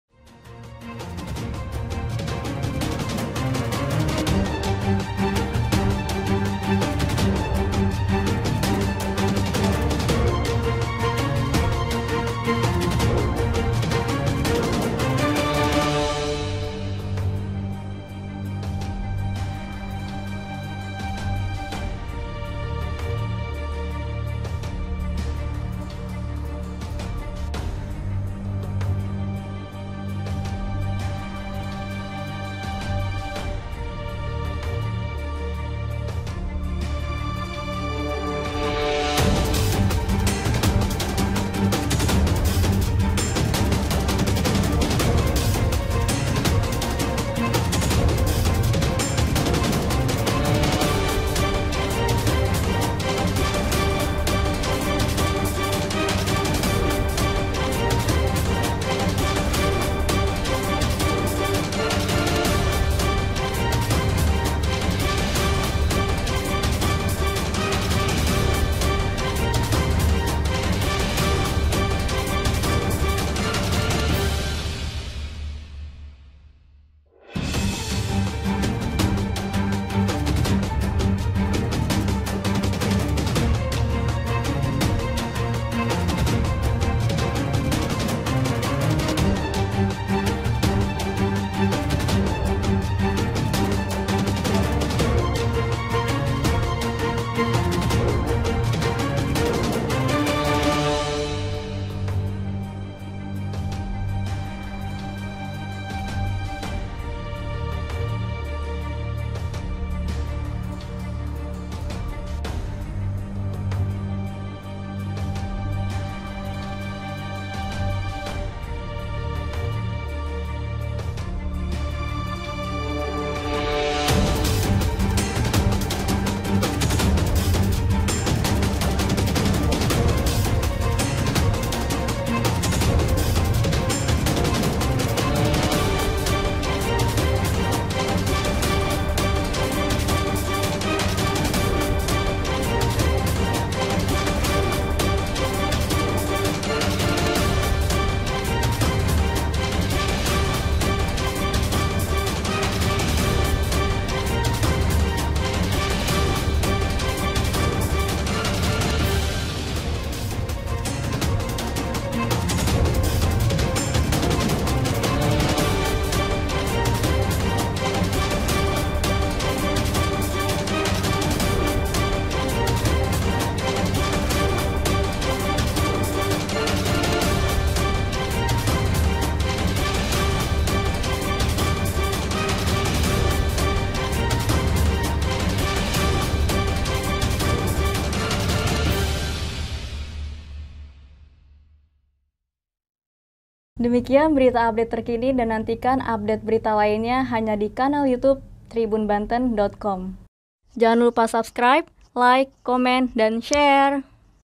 Kondisi Terkini Bocah 3 Tahun di Banten Suspect Hepatitis Akut
TRIBUNBANTEN.COM - Kepala Dinas Kesehatan (Dinkes) Provinsi Banten, Ati Pramudji Hastuti, membenarkan adanya kasus hepatitis akut di Banten.

Dia menegaskan pasien hepatitis akut itu berdomisili di DKI Jakarta.

"Pasien berusia tiga tahun," katanya, mengutip percakapan grup pesan instan, Selasa (24/5/2022) malam.

Menurut dia, pasien didiagnosa suspec hepatitis akut oleh sebuah rumah sakit di Kota Tangerang.

Saat ini pasien masih dirawat di rumah sakit tersebut.

Sumber: Tribunbanten.com
Penulis: Ahmad Tajudin
Video Production : Nanang Purnadi